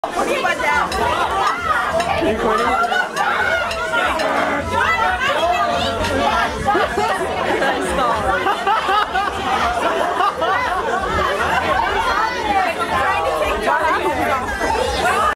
Dancing up on Tamiko
Vanessa's 7 layer party.
I was dared.
I complied.